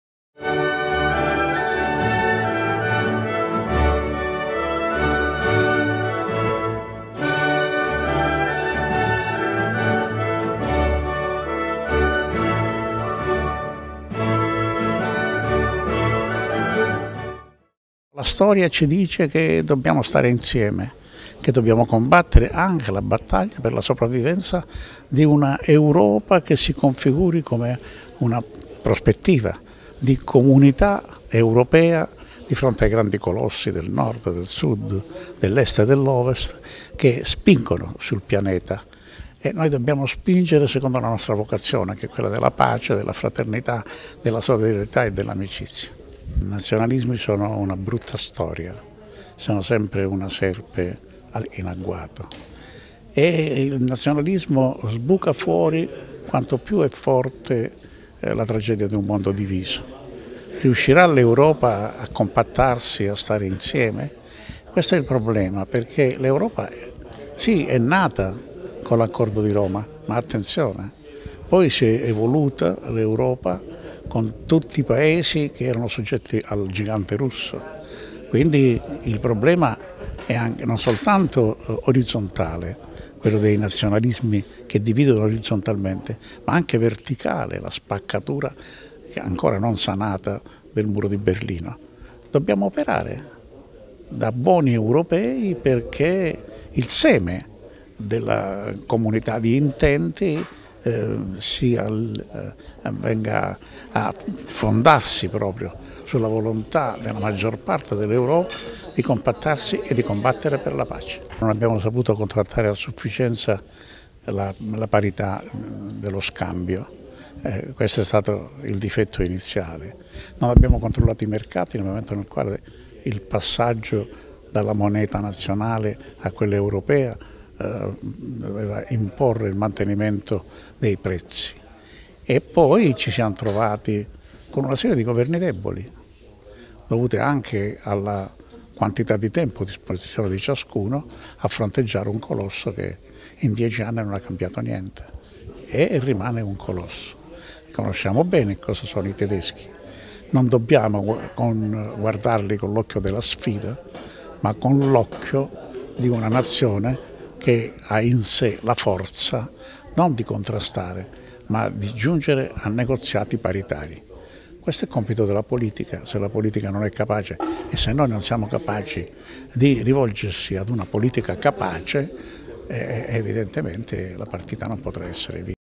Intervista On Armando Veneto
Intervista all'On. Avv. Armando Veneto, relatore del convegno "A 25 dal Trattato di Maastricht, Europa quo vadis?", sui problemi che attanagliano la zona euro.